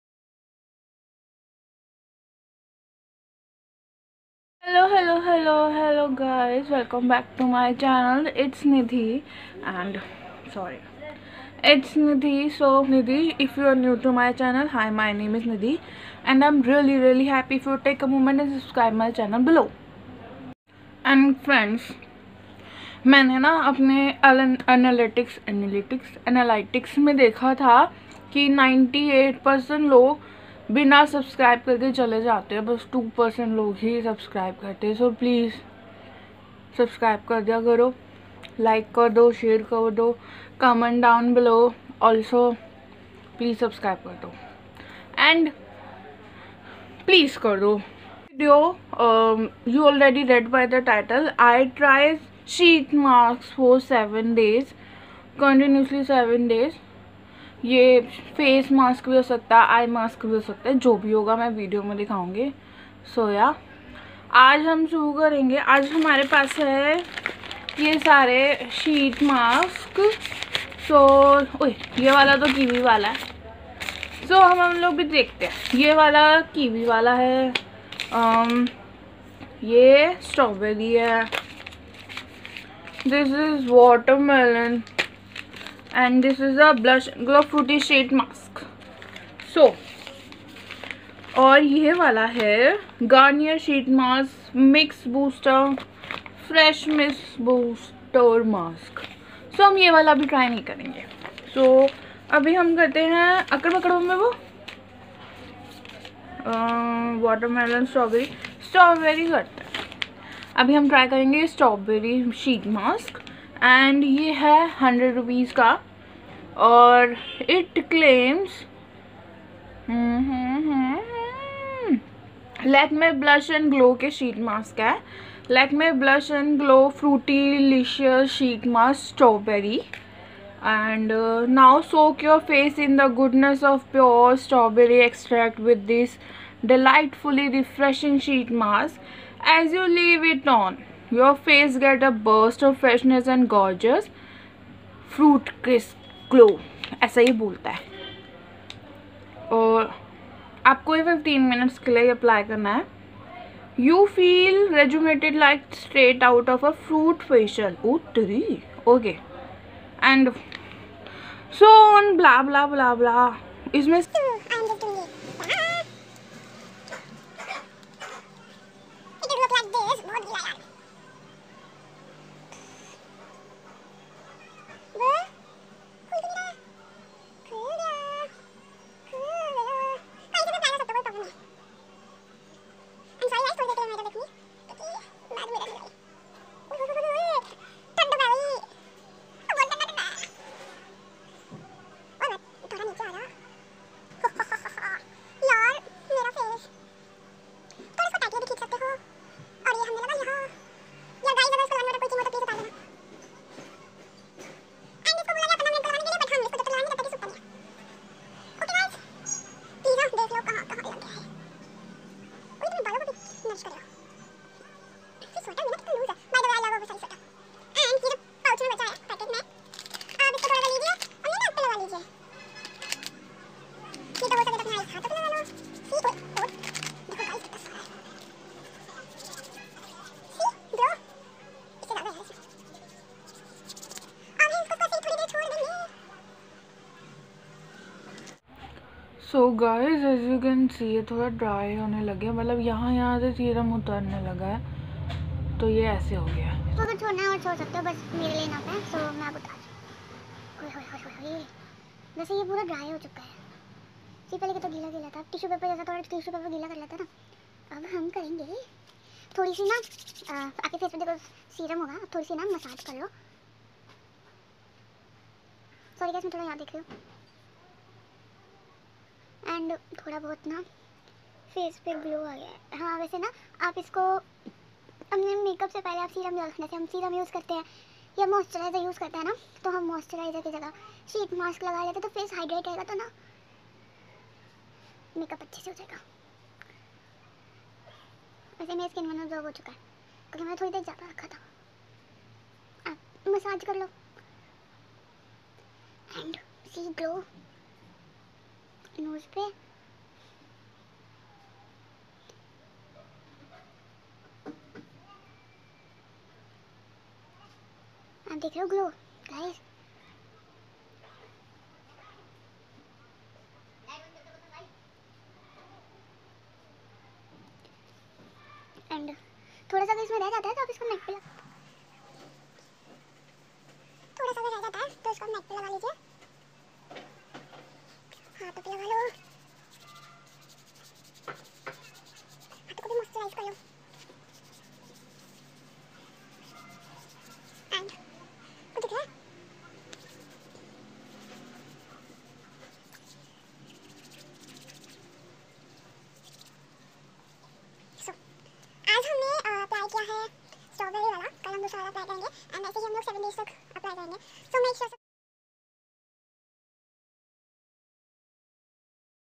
I tried SHEET MASK for next 7 days day 1|lakme blush&glow
I tried sheet mask for next 7days,it is lakme blush glow sheet mask flavour strawberry 🍓🍓sheetmask